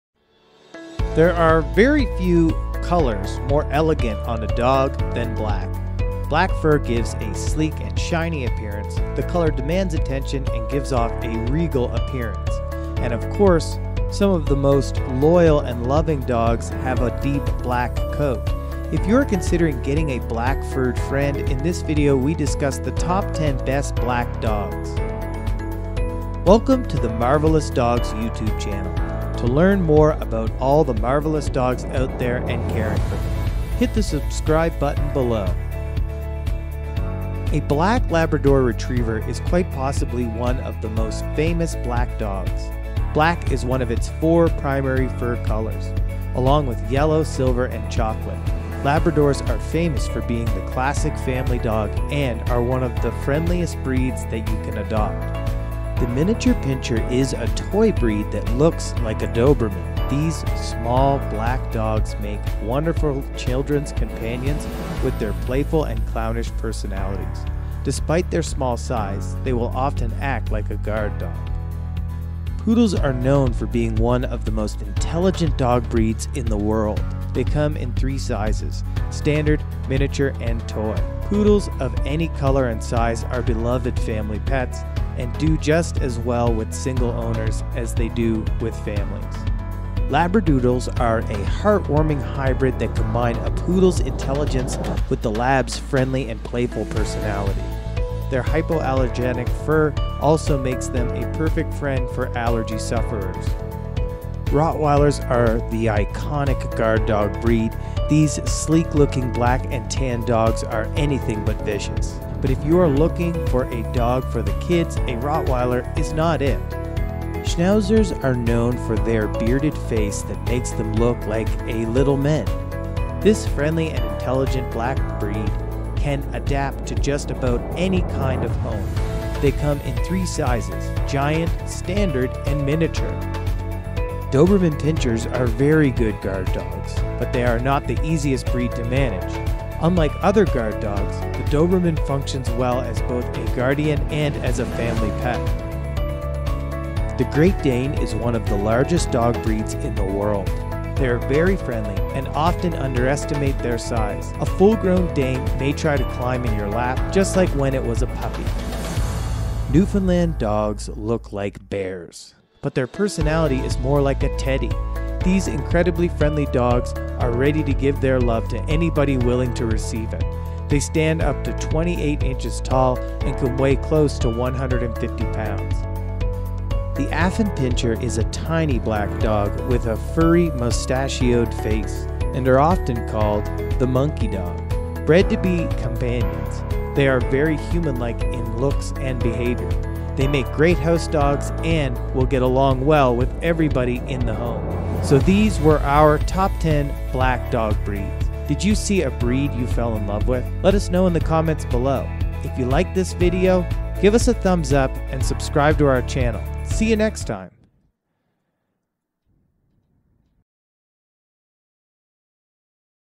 Black Dog Breeds: 10 Sleek And Shiny Dog Breeds You’ll Swoon Over!
Let’s be honest, there are adorable dogs in every color. But there is something about the smooth and shiny coat of Black dog breeds that instantly draws the eye. And yet they are often neglected in adoptions due to their negative reputation. In this video, we choose 10 black dog breeds that are the best companions you could hope for.

A black dog is no different than any other dog, and we urge you to not let their reputation hold you back, at the shelter. Some of the most popular dogs, such as the Labrador Retriever, Poodle and Schnauzer, come in beautiful black coats and their characteristic happy, loving personalities. And if you open your heart to one of these dogs, they will reciprocate with all that love and more!